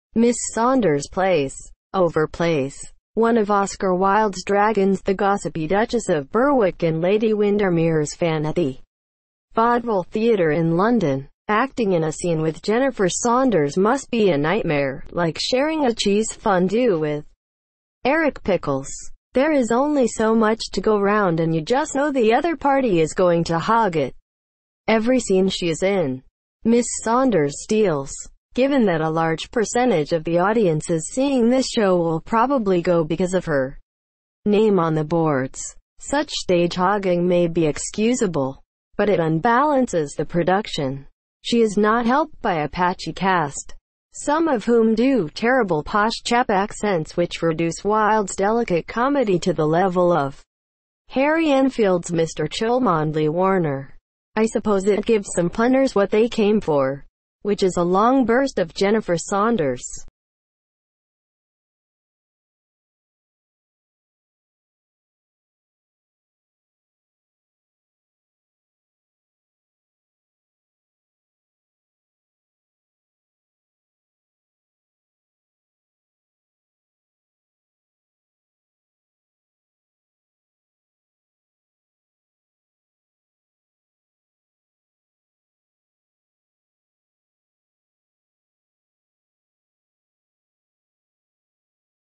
QUENTIN LETTS first night review of Lady Windermere's Fan ‍
[ Miss Saunders plays (overplays) one of Oscar Wilde’s dragons – the gossipy Duchess of Berwick in Lady Windermere’s Fan at the Vaudeville Theatre in London. ,  Acting in a scene with Jennifer Saunders must be a nightmare – like sharing a cheese fondue with Eric Pickles. There is only so much to go round and you just know the other party is going to hog it. ,  Every scene she is in, Miss Saunders steals. Given that a large percentage of the audiences seeing this show will probably go because of